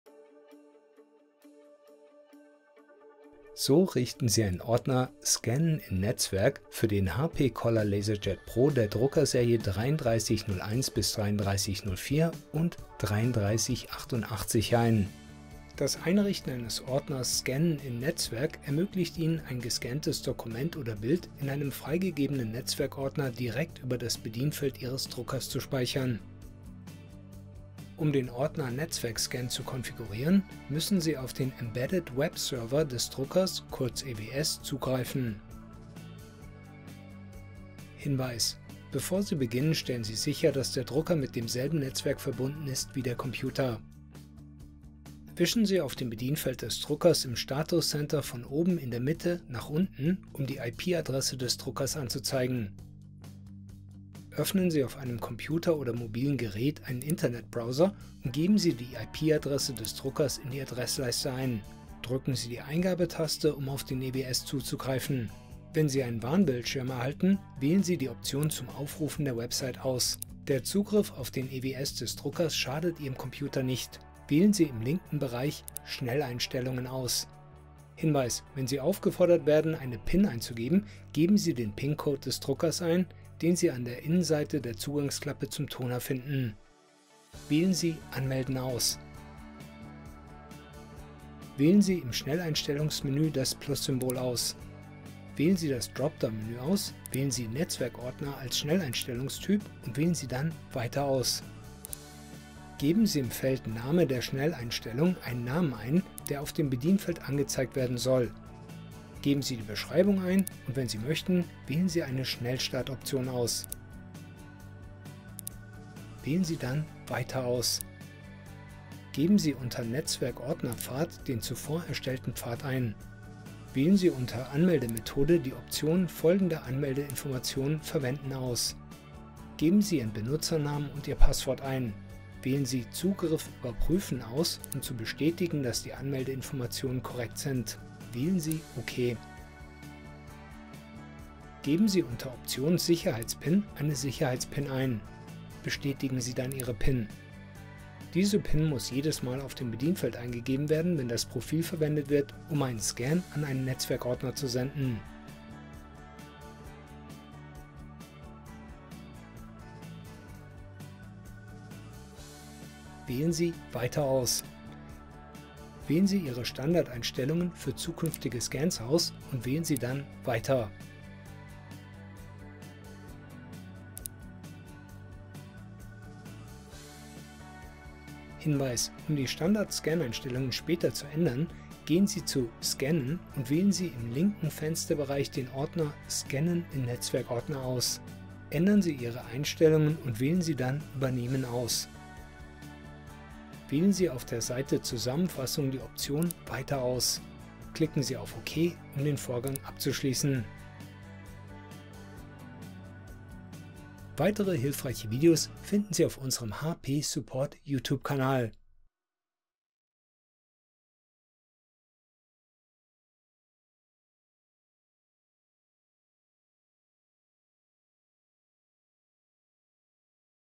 Ordner „Scannen an Netzwerk“ einrichten | HP Color LaserJet Pro 3301-3304, 3388 Drucker | HP Support
Durch das Einrichten eines Ordners „Scannen an Netzwerk“ können Sie ein gescanntes Dokument oder Bild direkt über das Bedienfeld Ihres Druckers in einem freigegebenen Netzwerkordner speichern. Erfahren Sie, wie Sie diese Funktion mithilfe des Embedded Web Server für Ihren HP Color LaserJet Pro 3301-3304 oder 3388 Drucker einrichten.

Kapitel:
00:00 Einleitung
00:24 Zugreifen auf den Embedded Web Server des Druckers
01:05 Ermitteln der PIN des Druckers